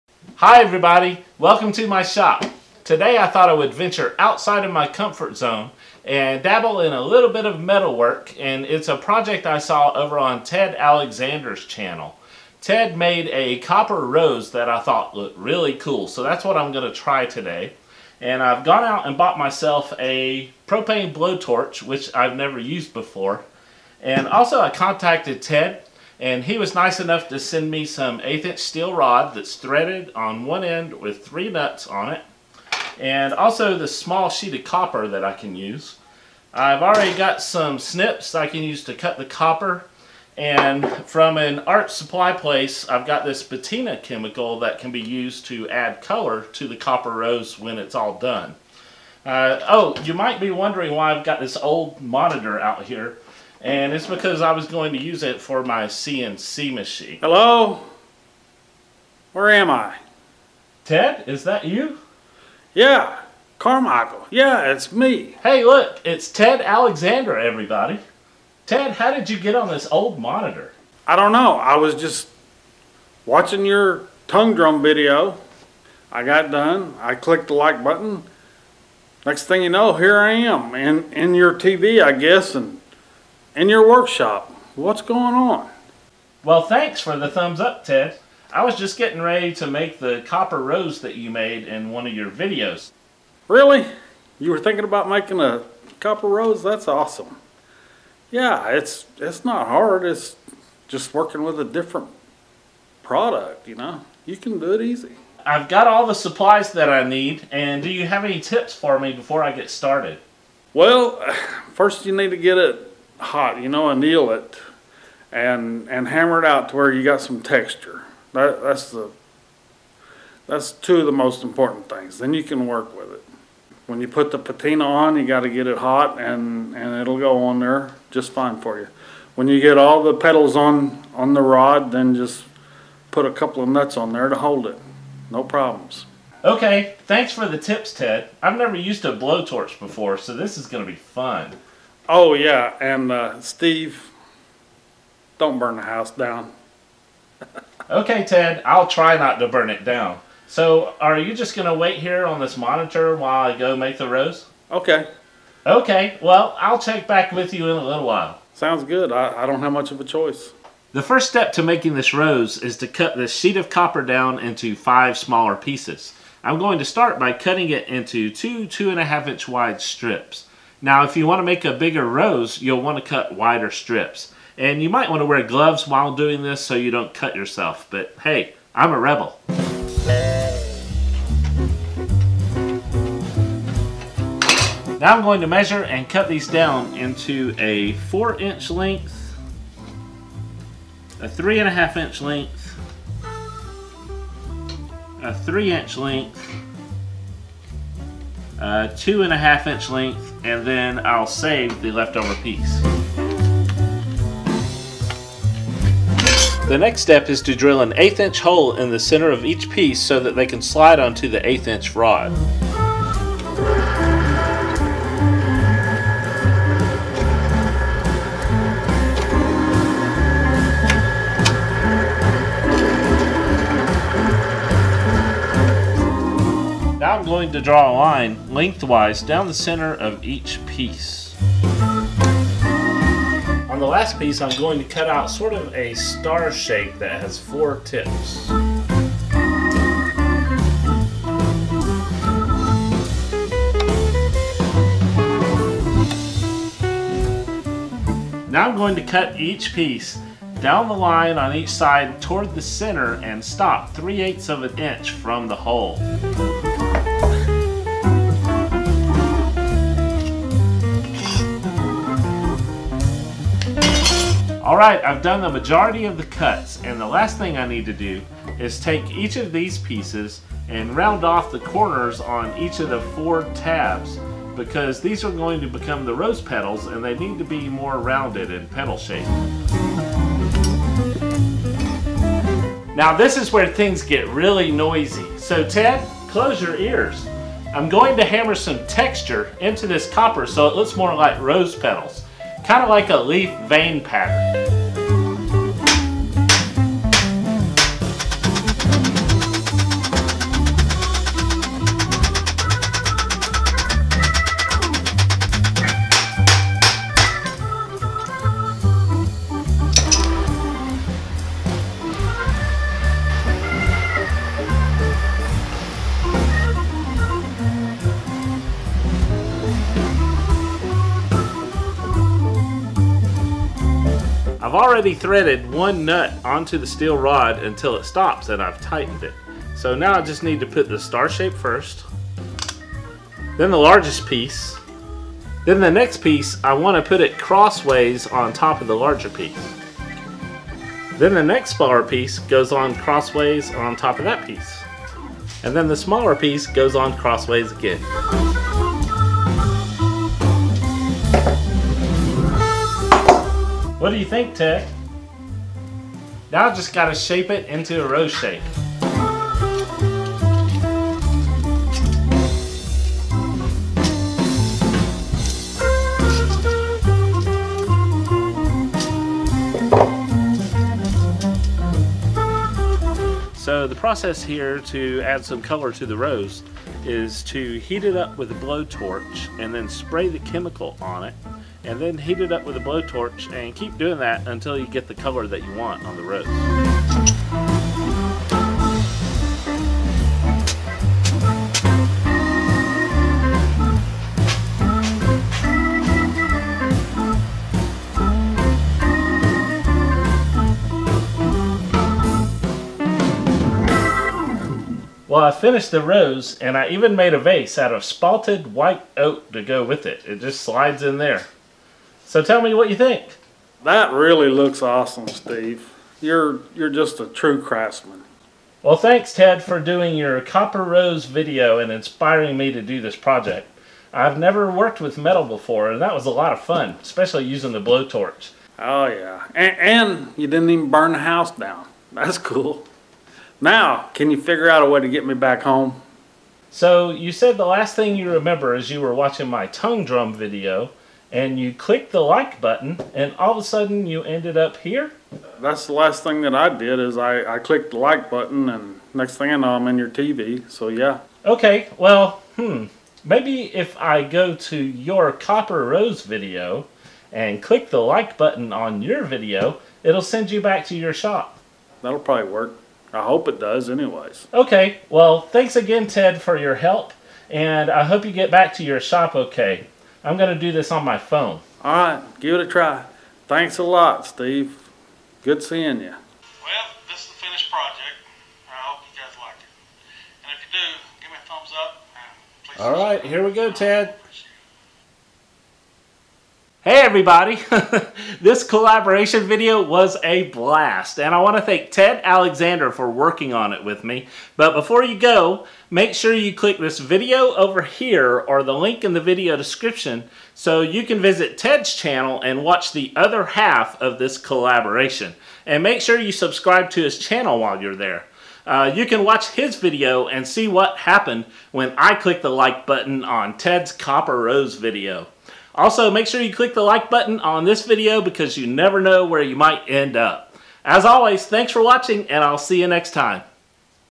Making a Copper Rose - Collaboration with Ted Alexander!
Ted Alexander and I decided to swap projects in this two-video collaboration.  I made one of Ted's copper roses and Ted made one of my tongue drums!